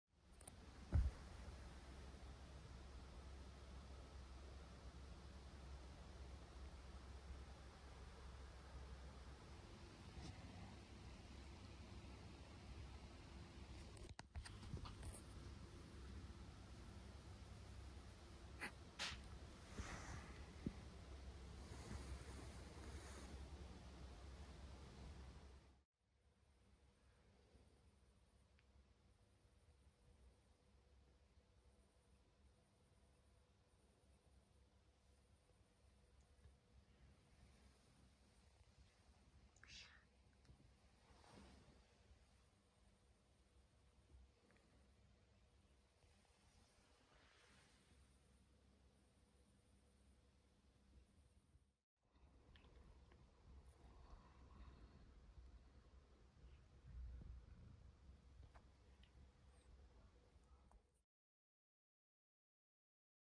"Appreciate the gesture, human"
Lucy the cat is really kind today - she let her human servant sleep on her pillow (but she still owns 90% of it). Have a great day! 😸